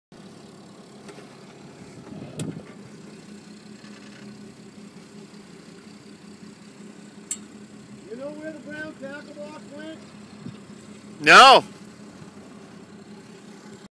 Massive school of mackeral on surface
We saw many schools of mackeral on the surface while out hinting bluefin tuna